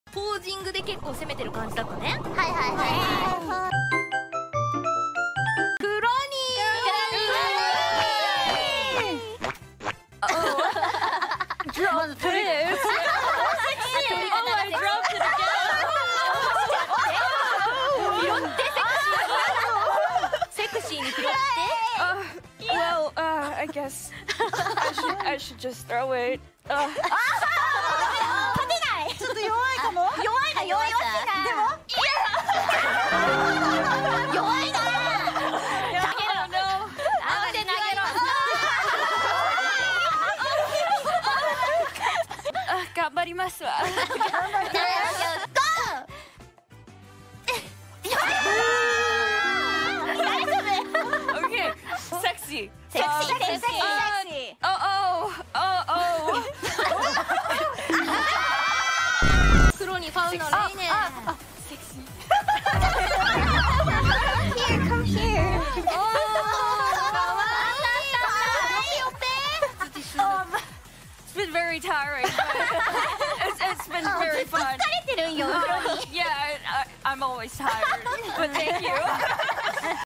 Kronii Being an Absolute Dork as She Attempts to be Sexy
god I love this dork so much

Source -【新水着お披露目】ドキッ！ホロライブだらけの水着コンテスト

Hololive HololiveEN HoloCouncil Kronii funny moments kronii clips kronii swimsuit